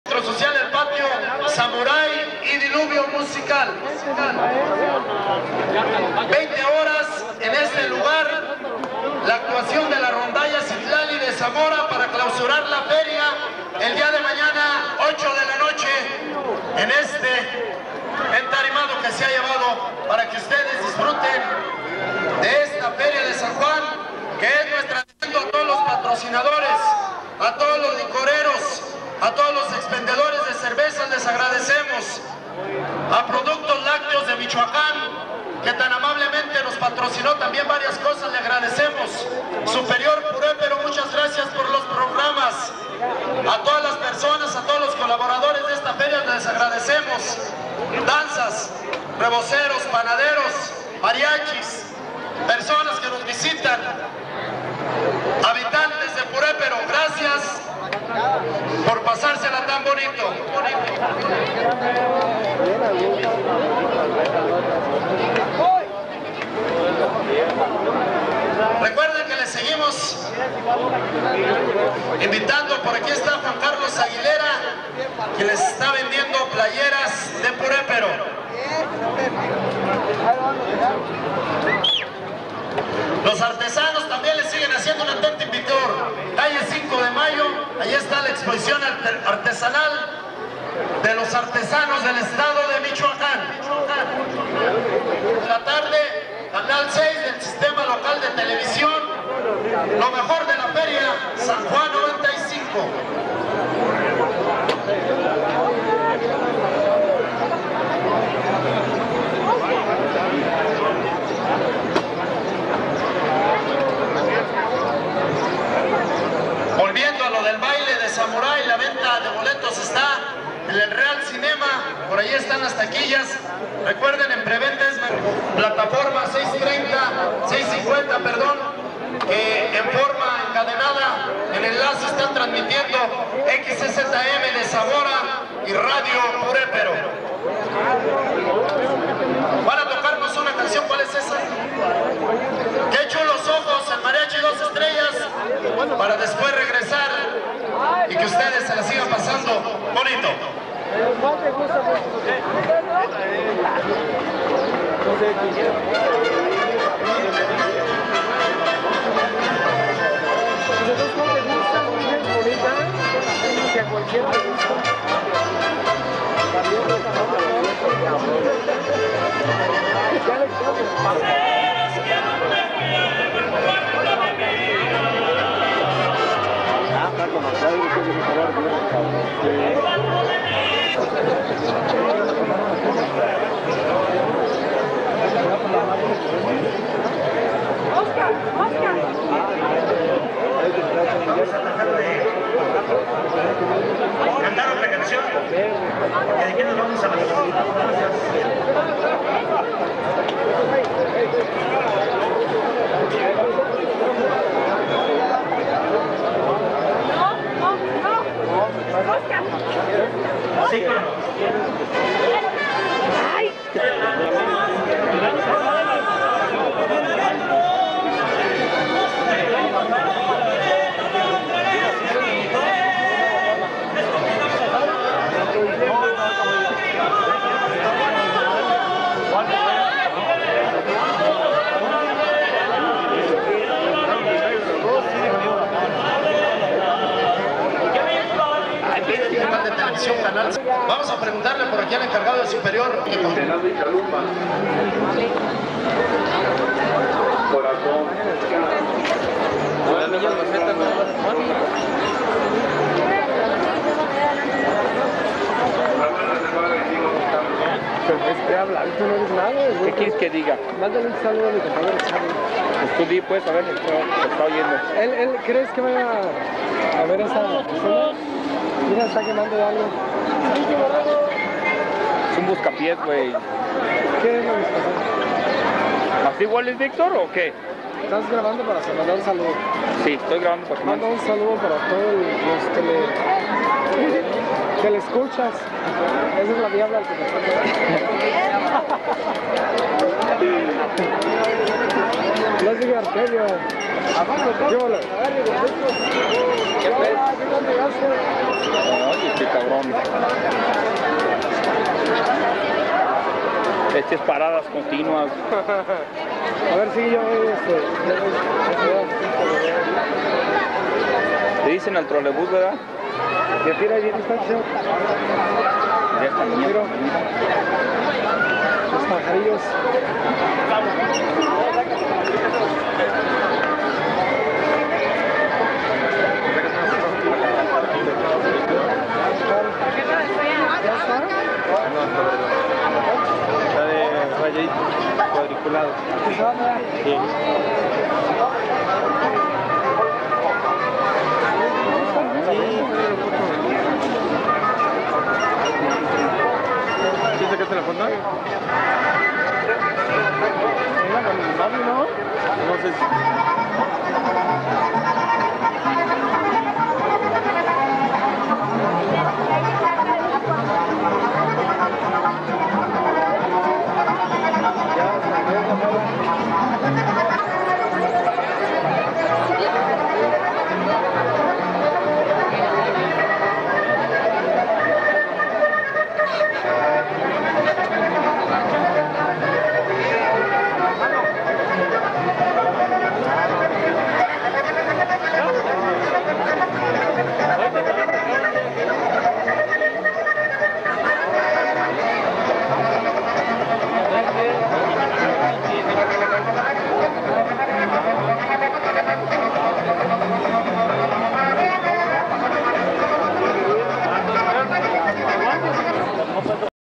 Purepero Michoacan 1995
Un video de la San Juan 1995, fue formato VHS y tratamos de que saliera lo mejor posible.